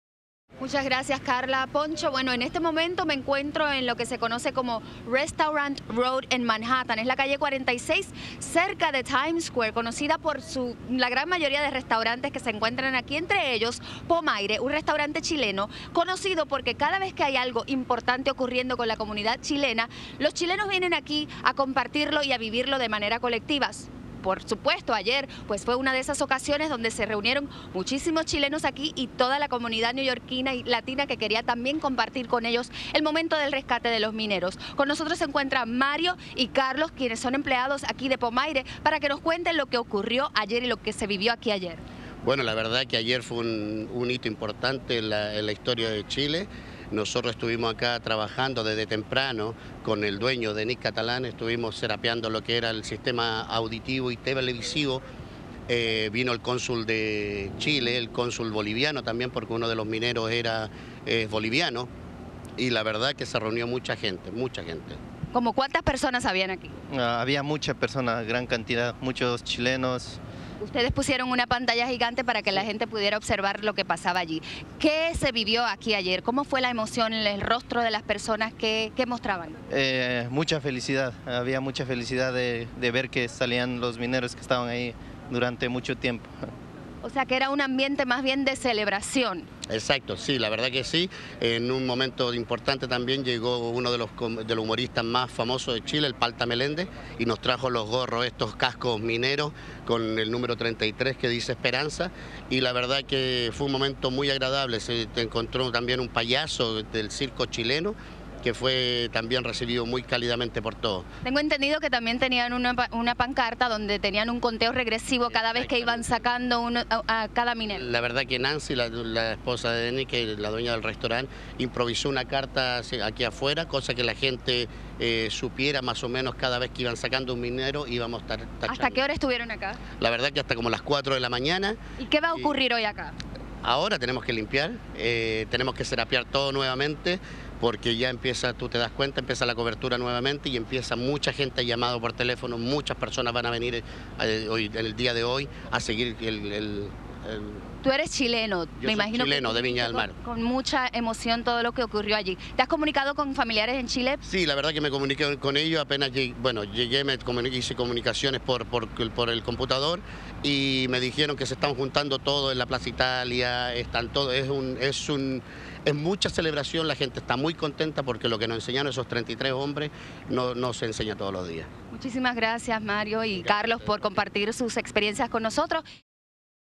Los chilenos celebran rescate en Nueva York
El rescate de los chilenos hace juntar a la comunidad chilena en la ciudad de Nueva York